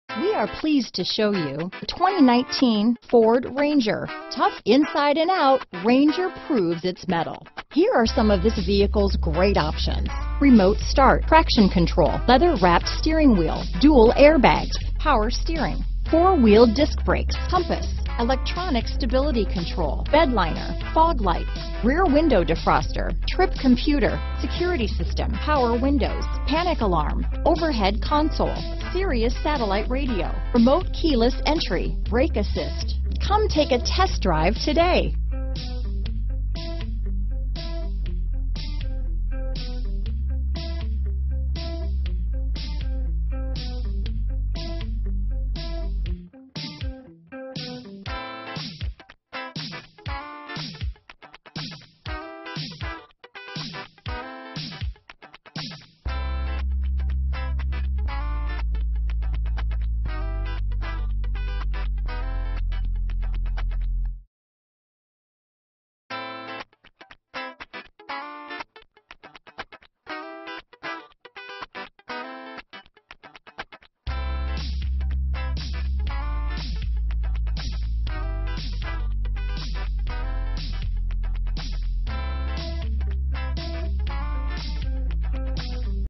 2019 Ford Ranger Ayer, Westborough, Winchendon, Auburn, Townsend, MA 528900
Magnetic N 2019 Ford Ranger available in Ayer, Massachusetts at Gervais Ford. Servicing the Westborough, Winchendon, Auburn, Townsend, MA area.

Gervais Ford
5 Littleton Rd